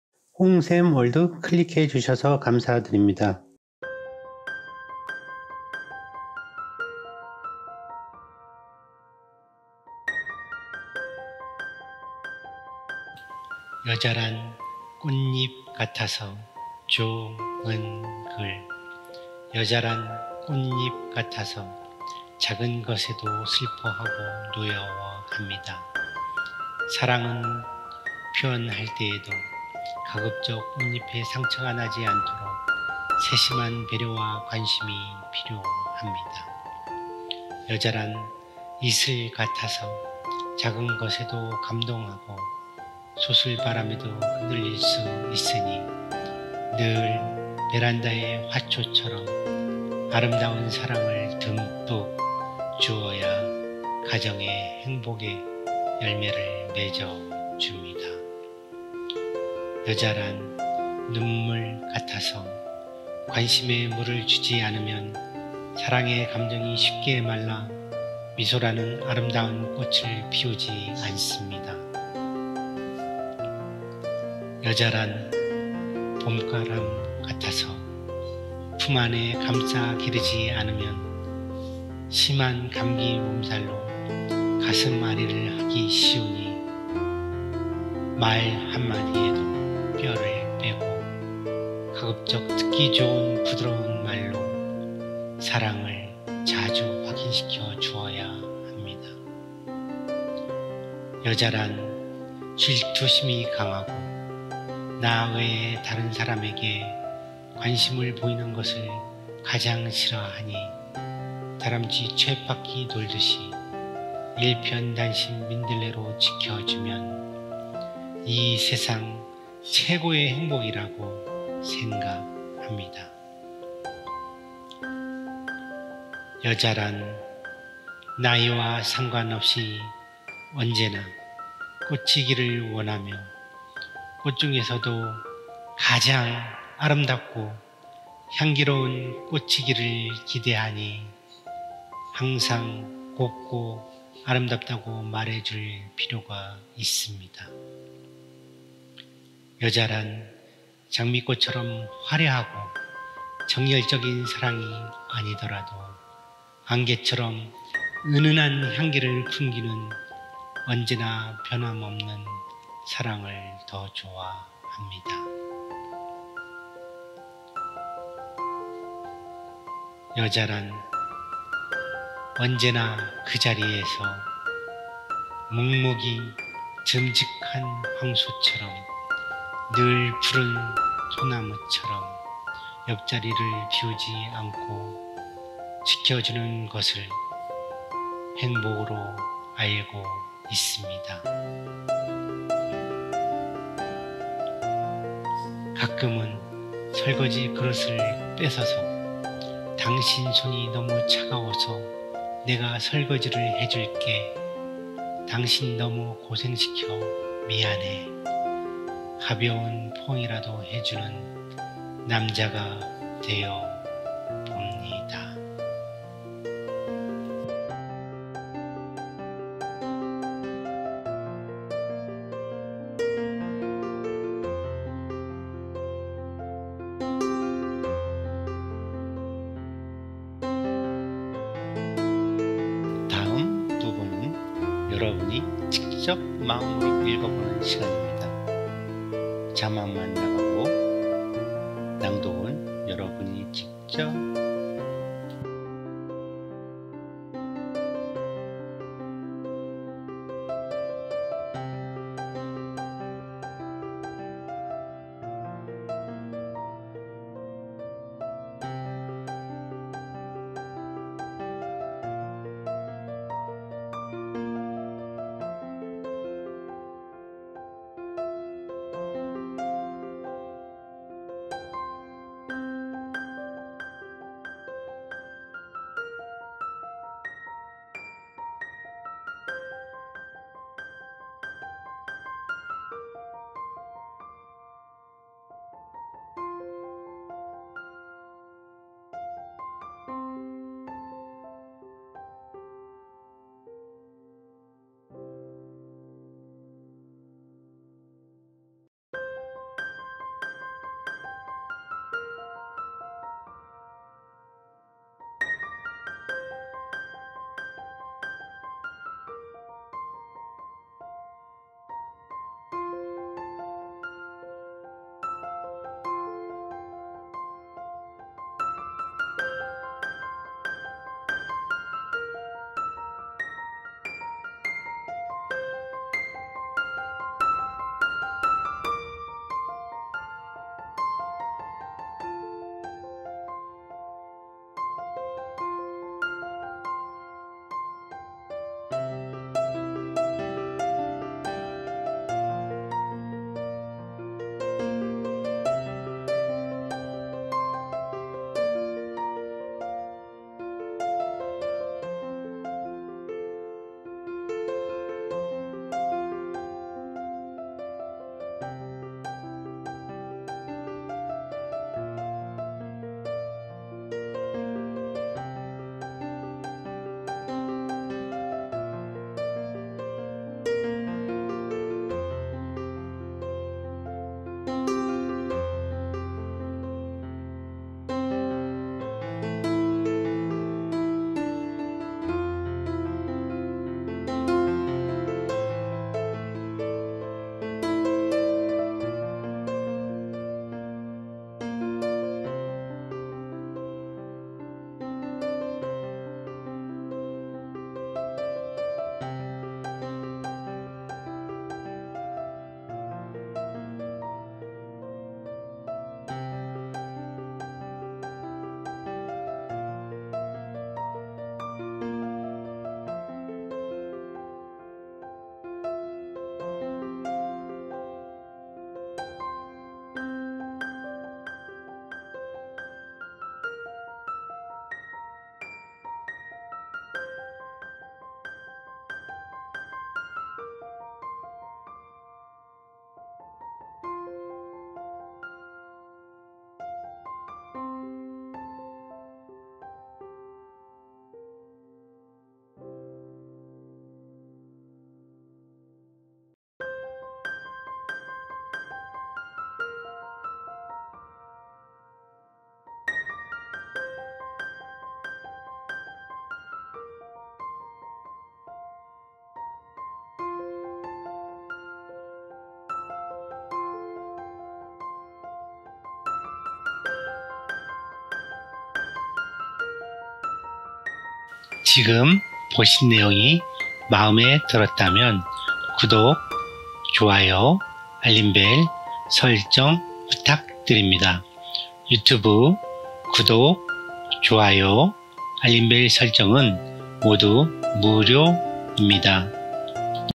좋은글, +여자란 꽃잎 같아서 / 좋은글, 사랑이 그리운 시
좋은글,
사랑이 그리운 시,
+ 여자란 꽃잎 같아서 / 좋은글
여자란 나이와 상관 없이 언제나 꽃이기를 원하며
꽃 중에서도 가장 아름답고 향기로운 꽃 이기를 기대하니
항상 곱고 아름답다고 말해 줄 필요가 있습니다.